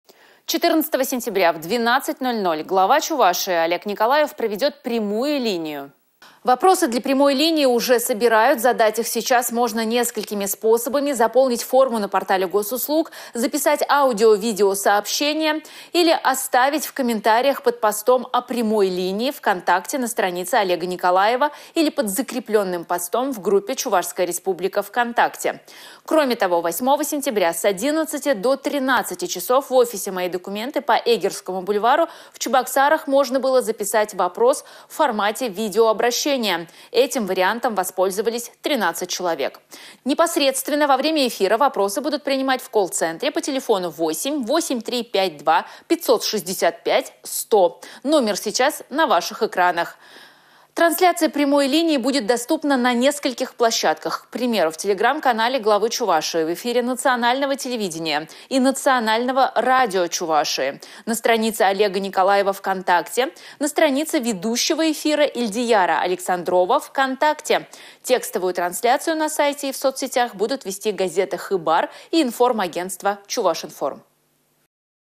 14 сентября Глава Чувашии Олег Николаев проведет прямую линию
14 сентября в 12:00 Глава Чувашии Олег Николаев проведет прямую линию.
Вопросы для прямой линию уже собирают. Задать их сейчас можно несколькими способами: заполнить форму на портале Госуслуг, записать аудио-, видеосообщение или отправить фото в комментариях под  постом о прямой линии «В контакте» на странице  Олега Николаева или под закрепленным постом в группе «Чувашская Республика» «Вконтакте».
Кроме того 8 сентября с 11 до 13 часов в офисе «Мои документы» по Эгерскому бульвару в Чебоксарах можно было записать вопрос в формате видеообращения. Этим вариантом воспользовались 13 человек.
Трансляция прямой линии будет доступна на нескольких площадках. К примеру, в телеграм-канале Главы Чувашии, в эфире Национального телевидения и Национального радио Чувашии, на странице Олега Николаева «Вконтакте», на странице ведущего эфира Элтияра Александрова «Вконтакте».